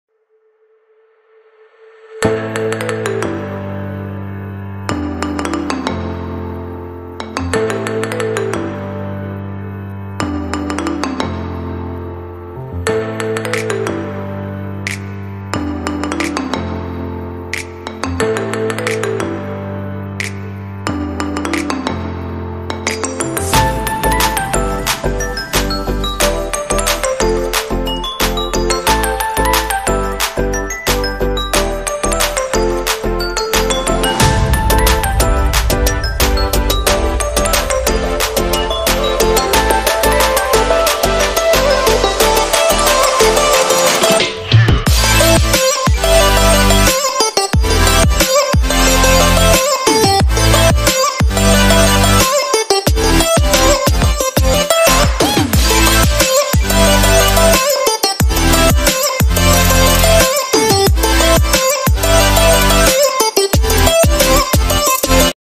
Paal payasam
Paal payasam is a south Indian dessert (a version of kheer), made during special occasions. It is a simple recipe with richness of nuts and goodness of milk.